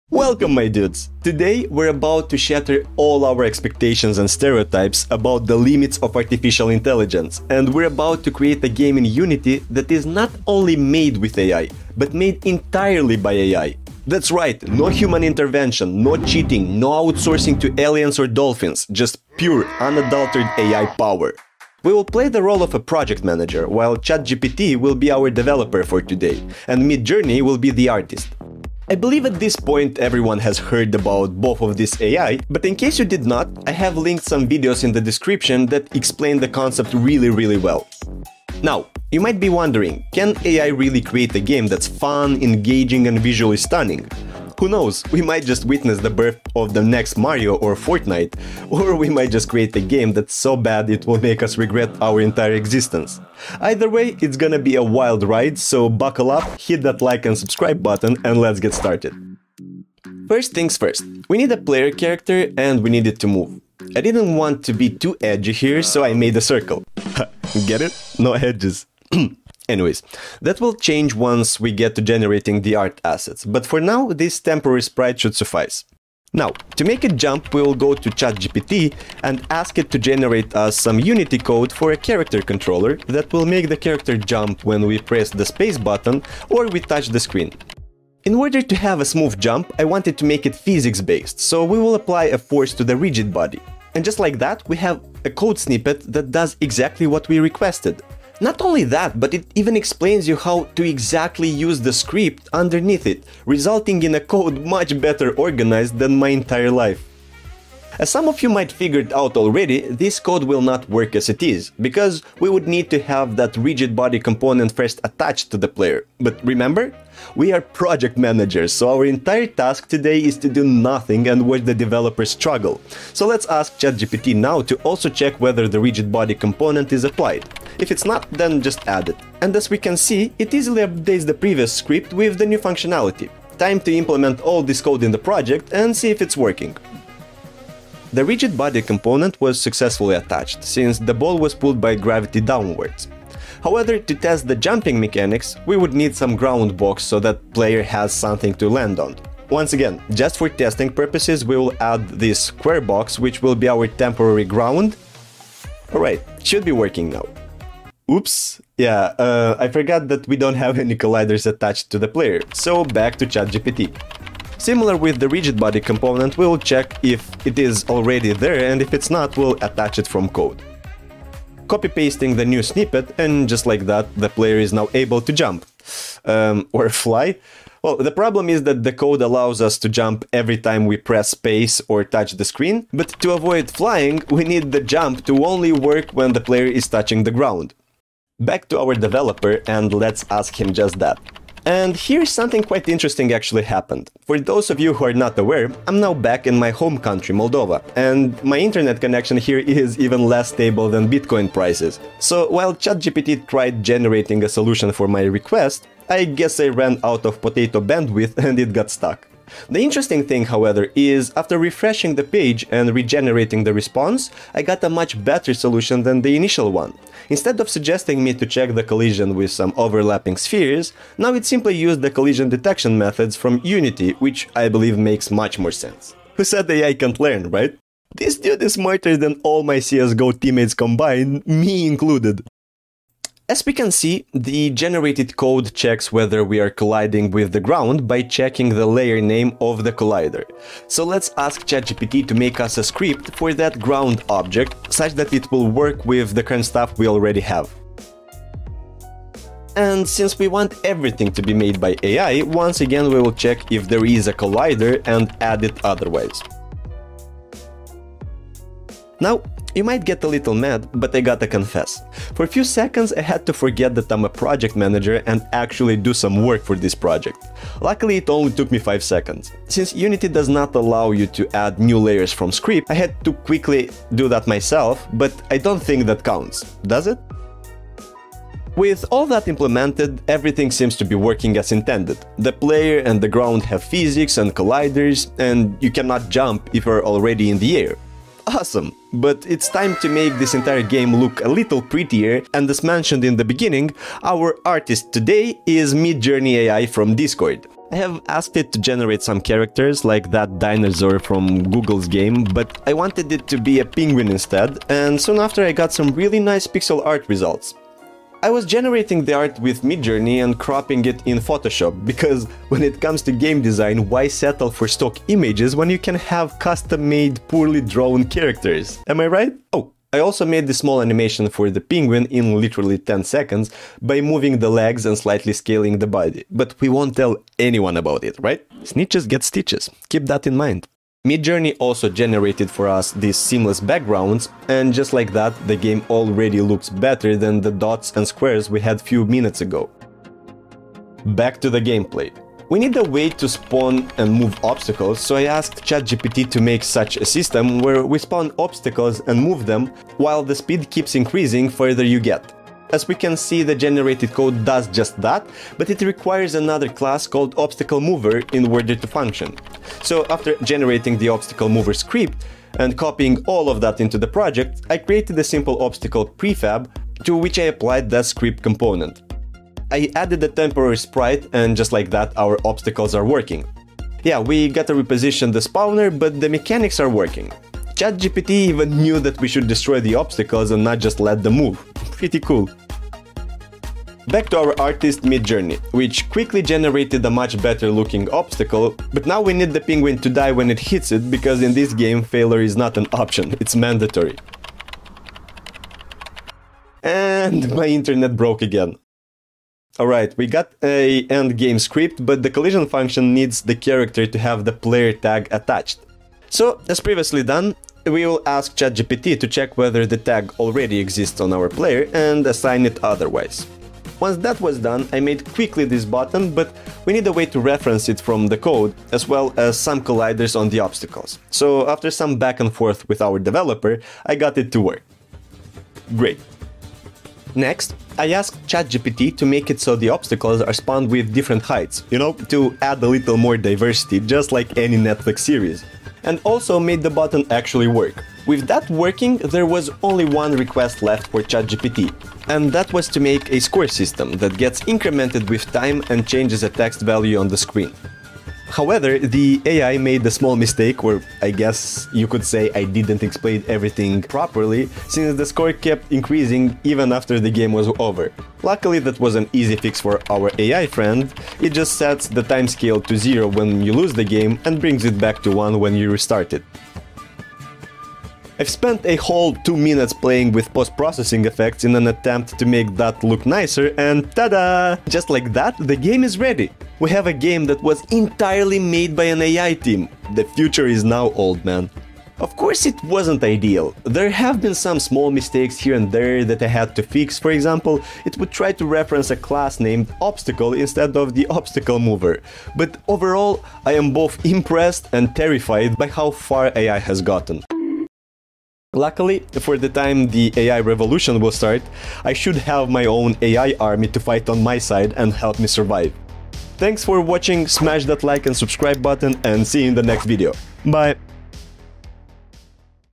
This Game was Made ENTIRELY by AI... (ChatGPT & MidJourney)
This ENTIRE game was made by A.I. ONLY!

This time, I come to you with a slightly unusual game dev video! This time I won't be developing the game myself, the game is FULLY made by AI! I am playing the role of a project manage, so I am doing literally nothing, while my AI team will struggle with the process. ChatGPT will be writing the code for the game, so it will play the role of the developer today and the art will be generated by MidJourney AI. Let's see what masterpiece can these 2 bring to life!

Time Stamps:
0:00 - Intro
1:00 - Player Physics and Movement
3:00 - Fixing Flying
4:00 - Ground Script
4:49 - Creating Assets with MidJourney AI
5:42 - Adding Obstacles
6:29 - End Game Functionality
7:18 - Score System
7:54 - End Result